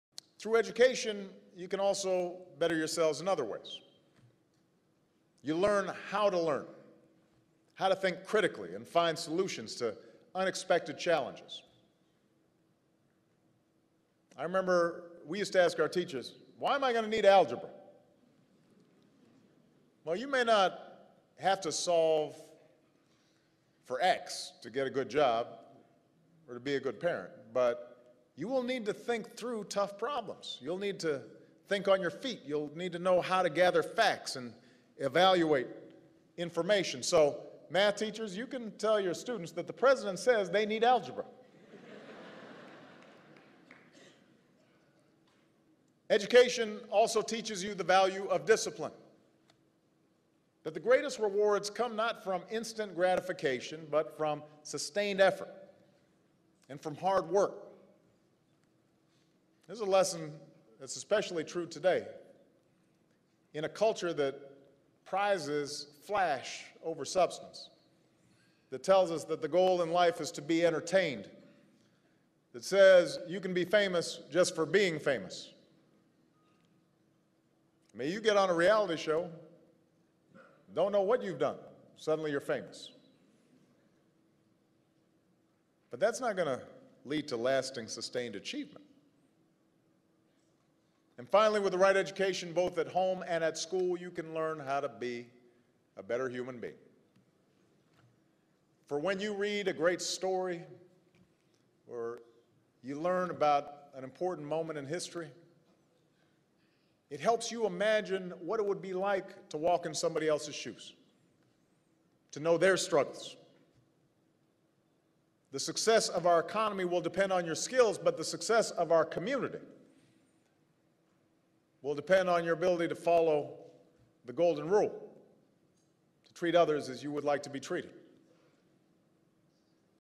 Importance of Education by American president
Ex President of United states highlight the importance of education on global summint